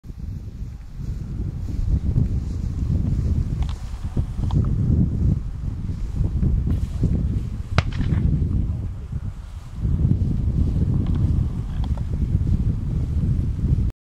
Memoriál Karla Podhajského 2010: dohledávka pernaté
Postřelení bažanta při dohledávce střelené pernaté zvěře v podání Heder Moon Eyes, vůdkyně Urazila Lichniak.